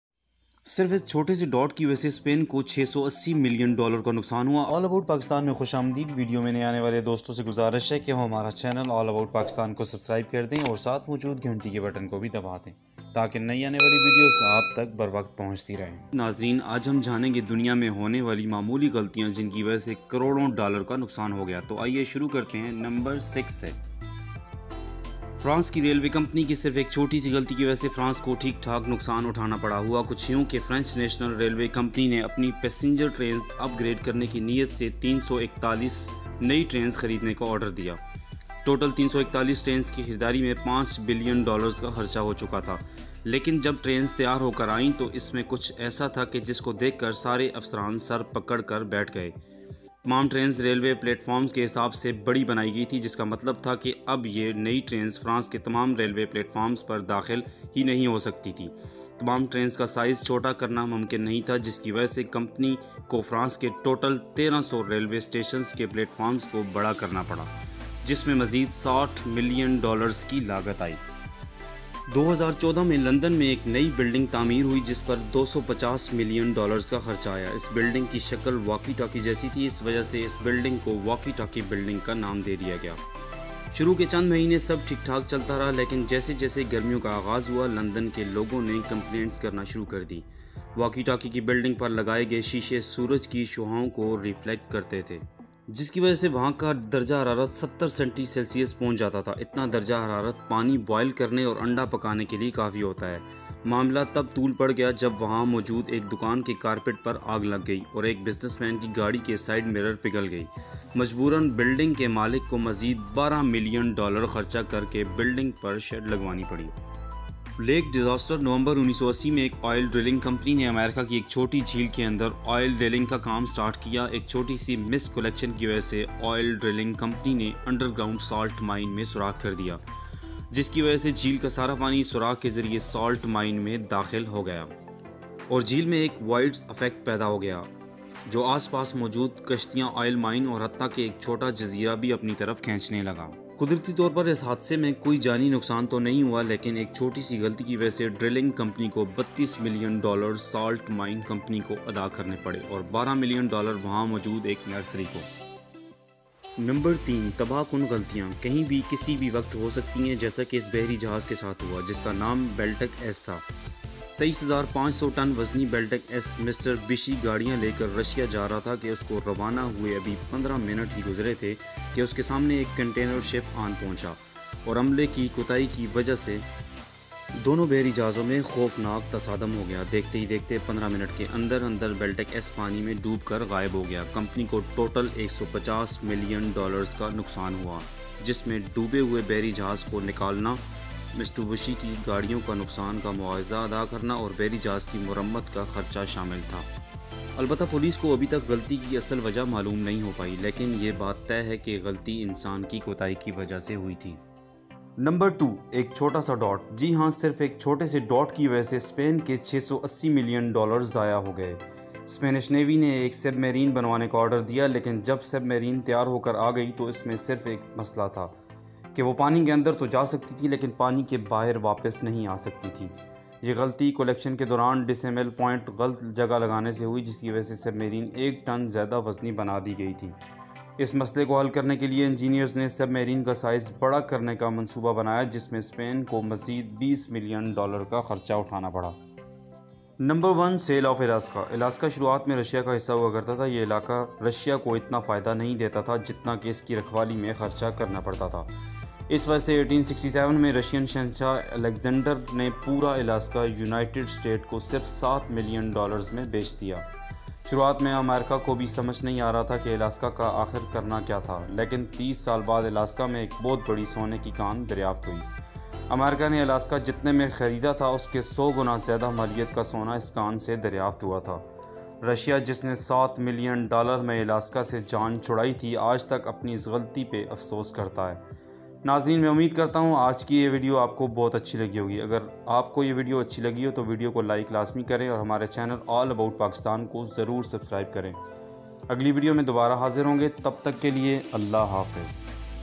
Minor mistakes in the world || One small dot cost Spain hundreds of millions of dollars
Title: One Small Dot Cost Spain Hundreds of Millions of Dollars | Shocking Mistake!

Description: Sometimes, even the smallest mistakes can have massive consequences! In this video, we dive into how a single dot (or decimal point) ended up costing Spain hundreds of millions of dollars. From accounting errors to astronomical miscalculations, learn how tiny details can lead to huge financial losses and disasters around the world.

Watch till the end to see how minor mistakes have changed the course of history!